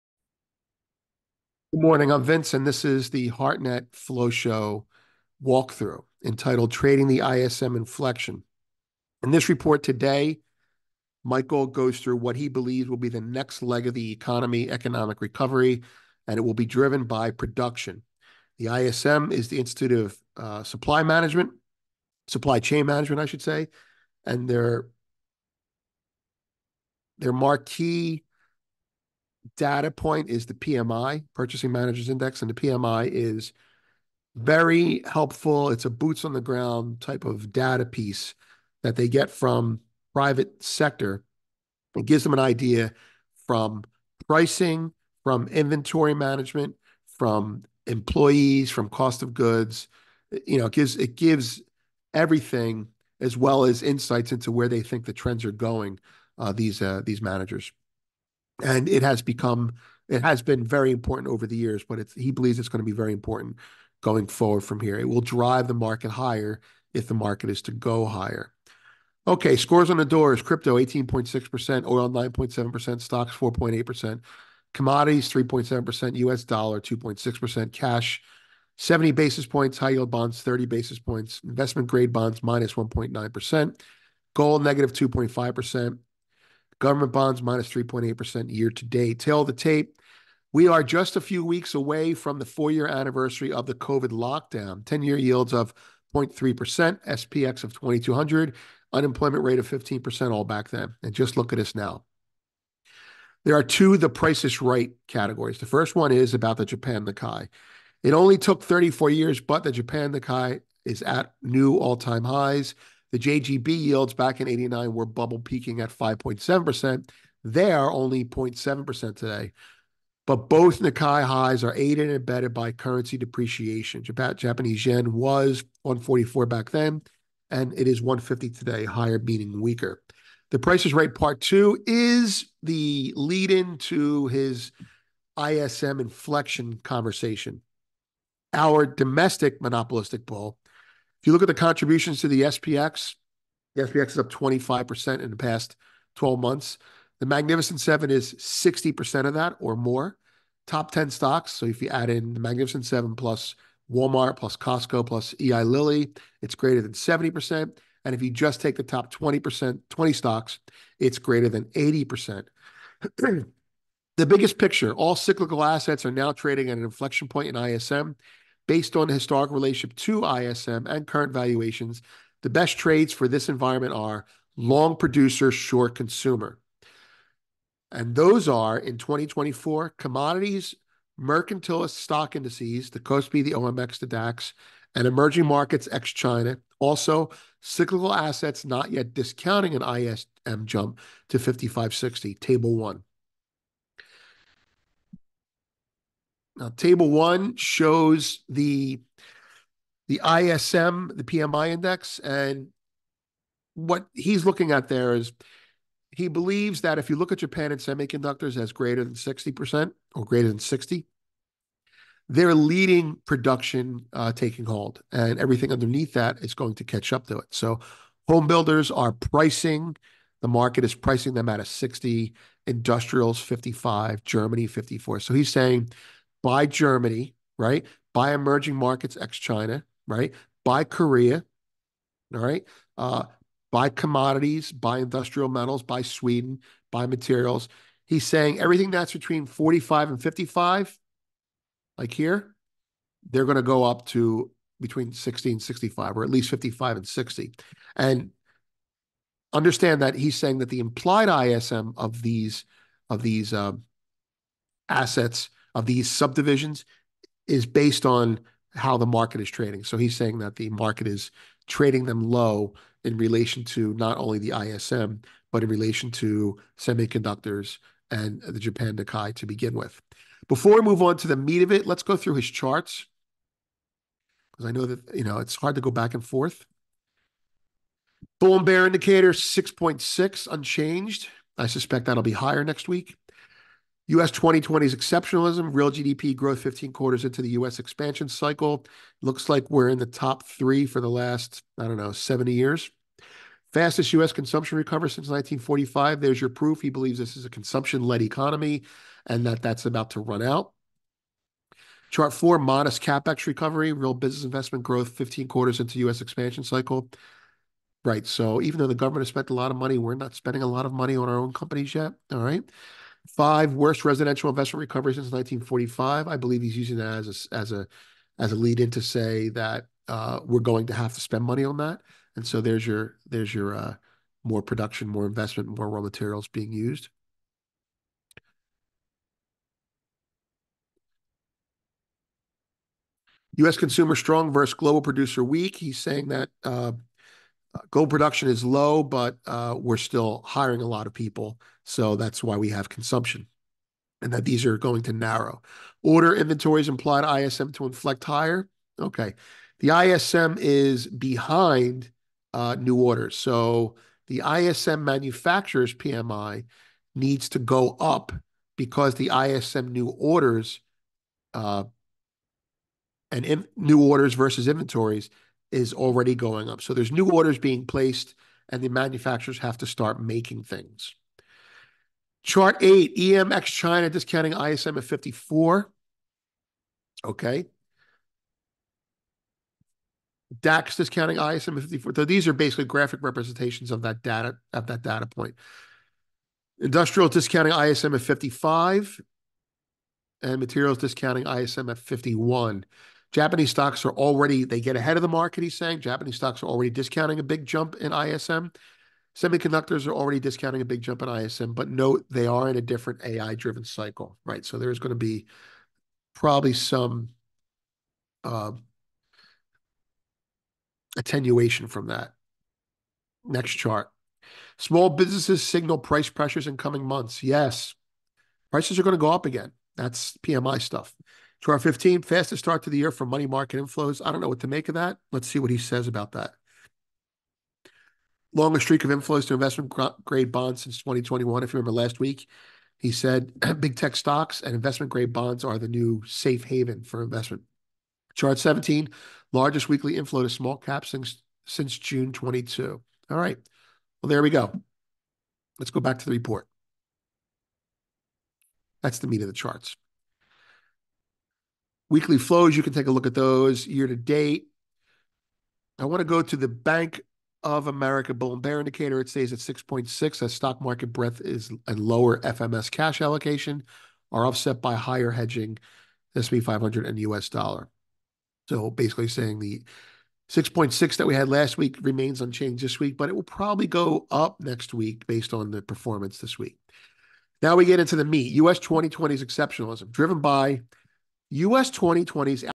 Hartnett: Here Comes Commodity Demand (PREVIEW)
Summary:
In today’s Flow Show Report, Michael Hartnett lays out why the US has done so well (American Exceptionalism) relative to global peers and then describes how the economy will change in the coming months from being consumer driven to being Producer driven. Finally, he touches on very broadly what he expects the priorities of Biden and Trump should either of them win the election in November. (18:47 Run time)

Timestamps:
    0:00- Intro: Covid lows, Japan, Mag 7
    4:30- Chart contextual walkthrough
    7:35- 3 Things Drive US Exceptionalism:
Report, transcript attached